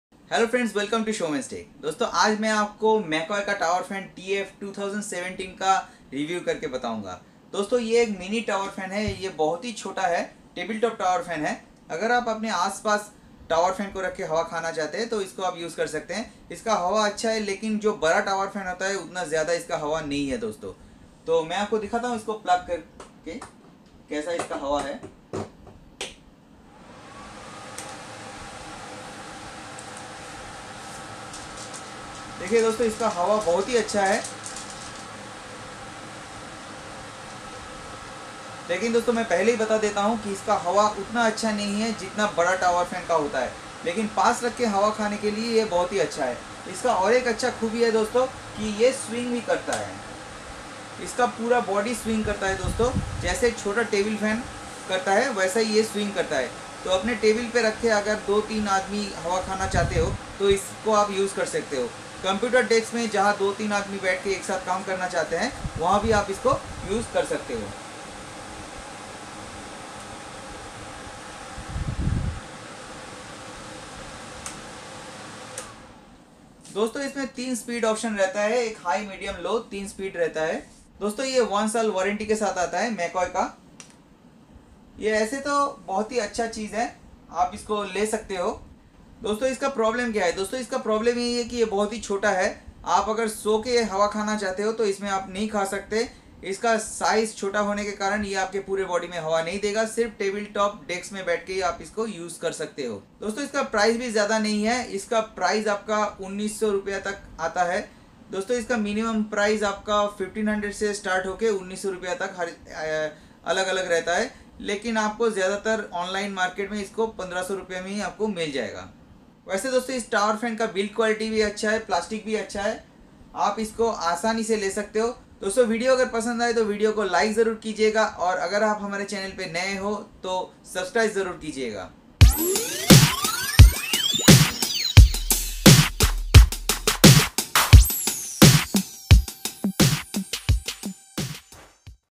Mini Tower Fan Reviews | Mccoy Rs 1500 Tower Fan Review | Tower Fans Online | By Soumens Tech
This video is all about McCoy mini tower fan reviews. McCoy launch new Rs 1500 tower fan. You can also buy tower fan online by click the link below at a discounted price.

Portable Mini Tower Fan with 90 Degree Rotating TF-104-B

Kumaka Portable Oscillating Mini Multipurpose Tower Fan